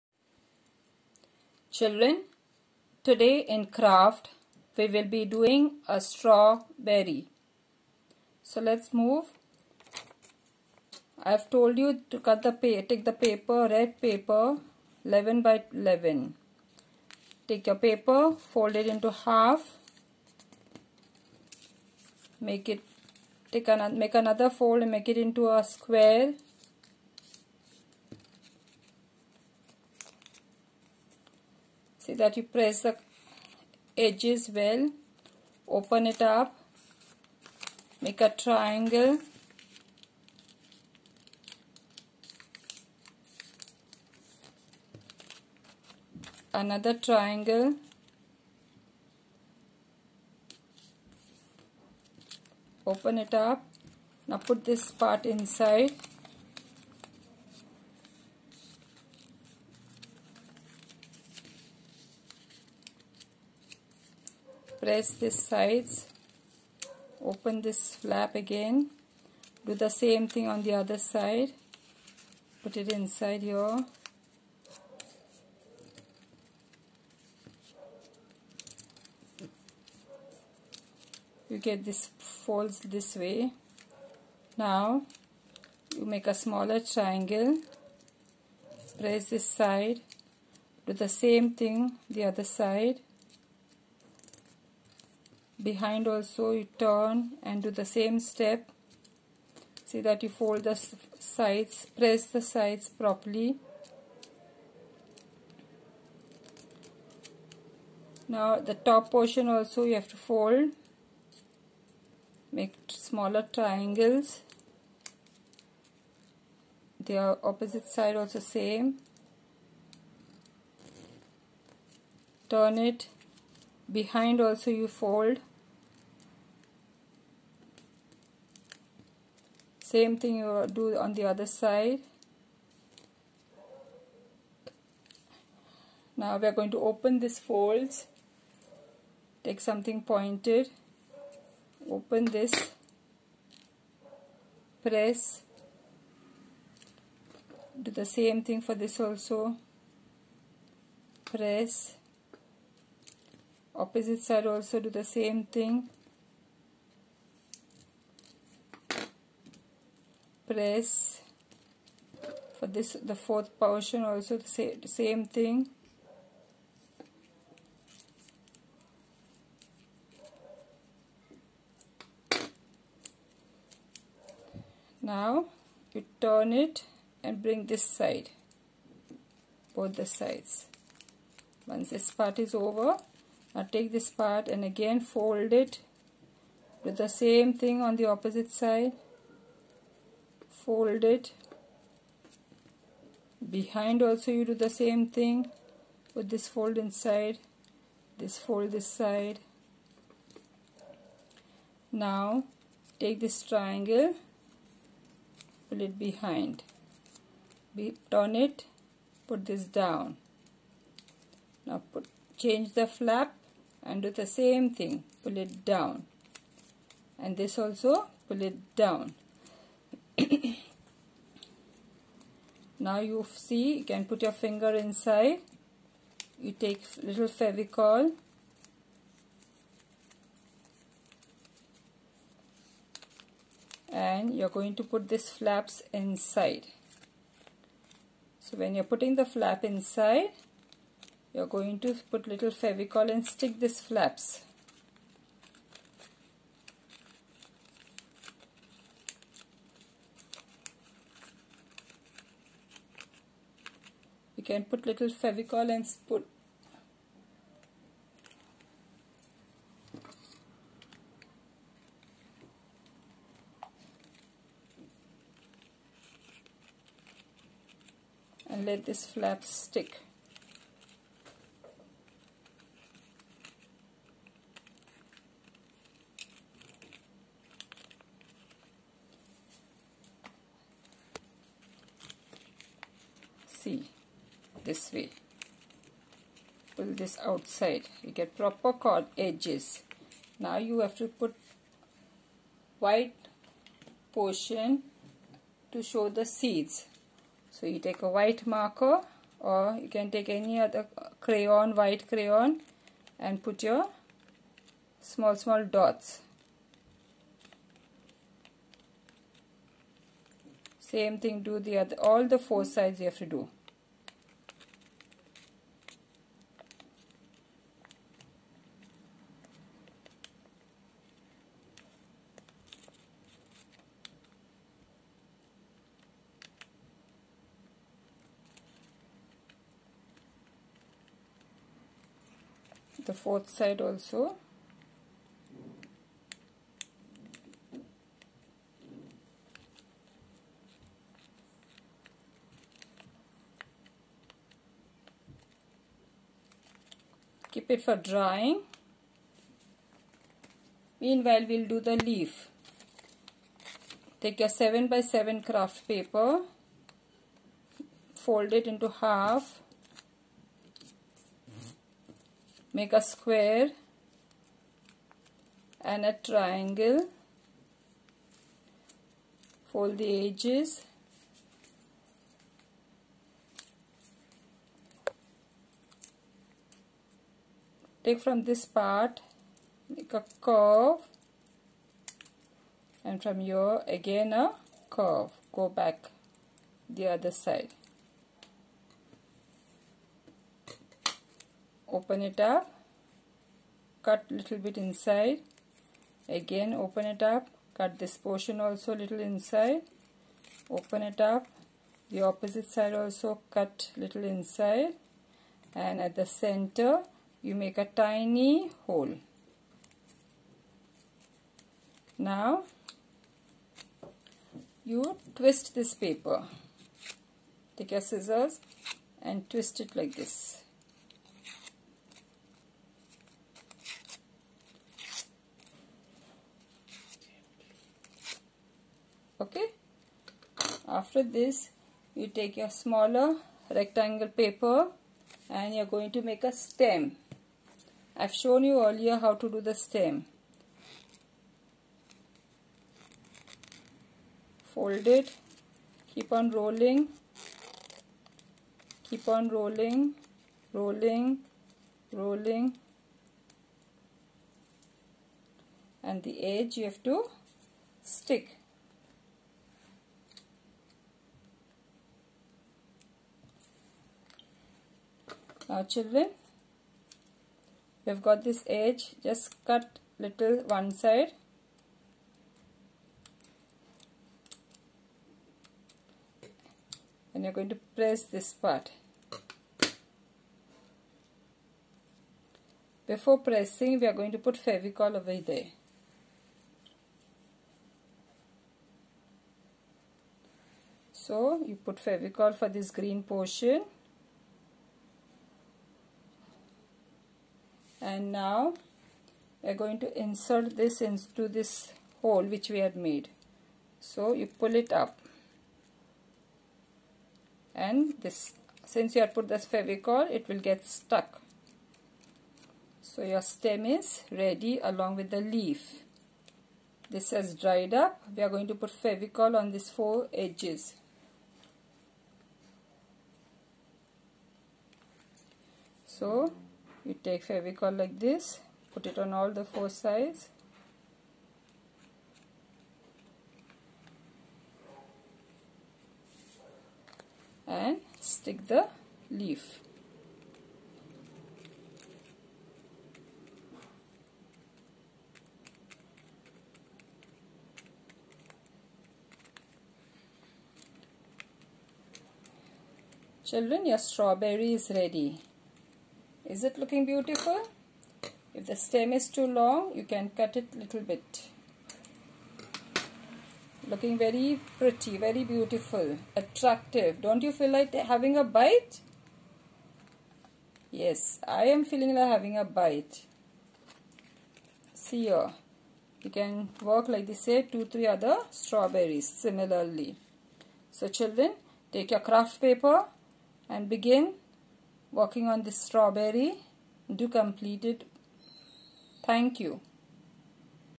Strawberry By Mrs. Joyce Gouria
This video is to help the children to make a strawberry